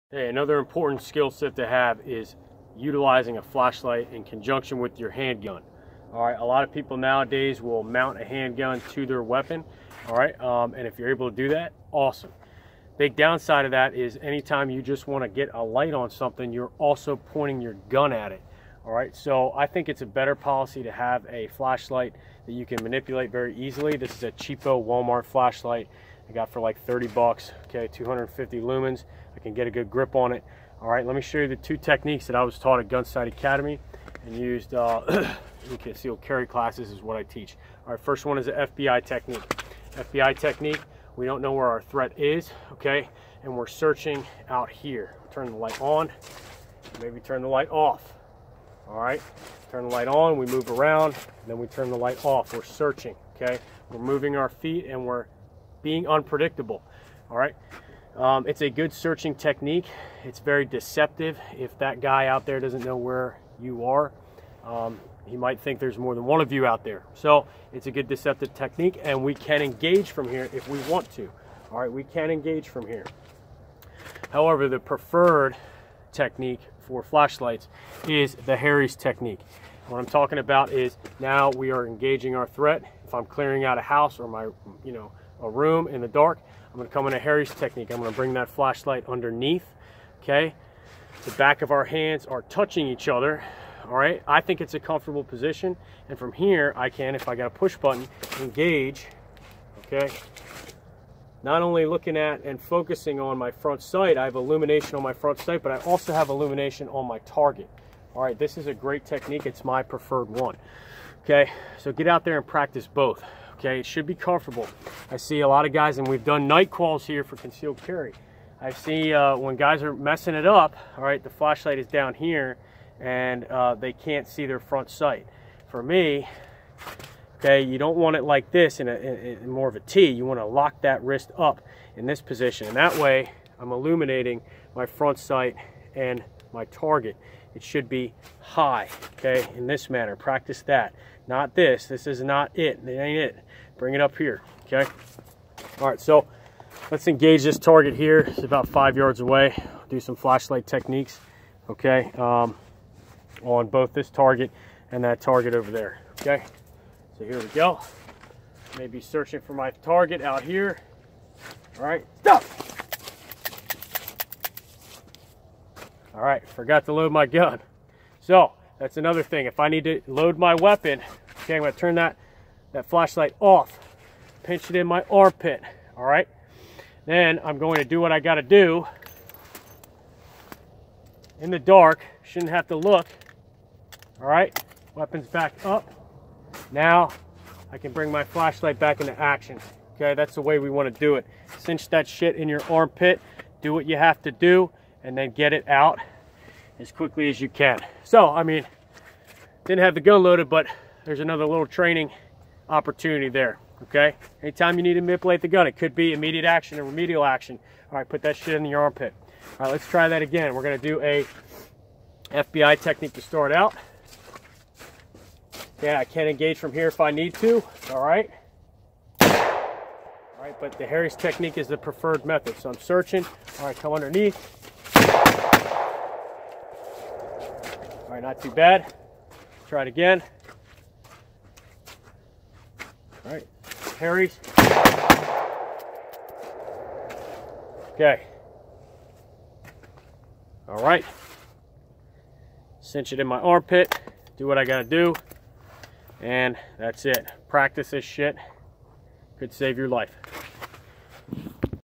Flashlight techniques for concealed carry
FBI techiqnique and Harries technique discussed when using a handgun